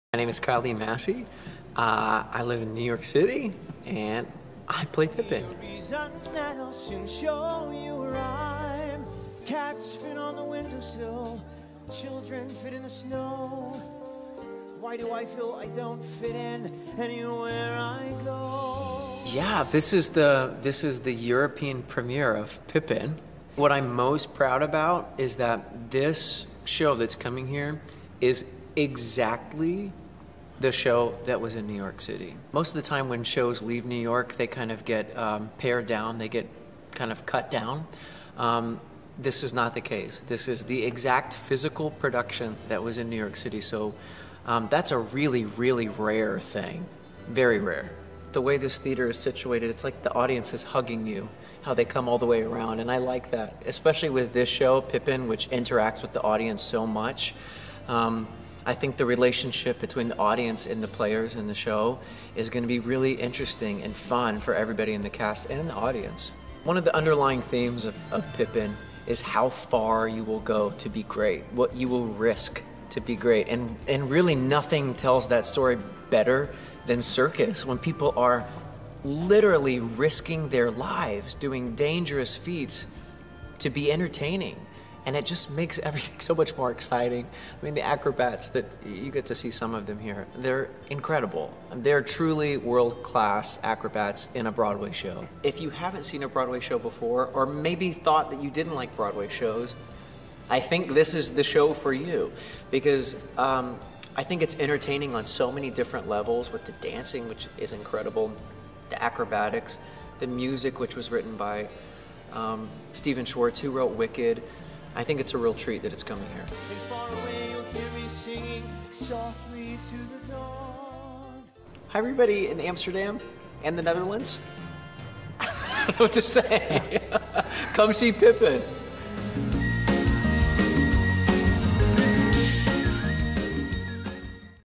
Kyle Dean Massey over PIPPIN in Carré
Kyle Dean Massey speelt de rol van Pippin in de gelijknamige Broawayshow. Hij vertelt ons graag alles over deze magische musical en waarom het zo bijzonder is dat deze in Nederland te zien is.

PIPPIN is van 9 maart t/m 10 april exclusief te zien in Carré.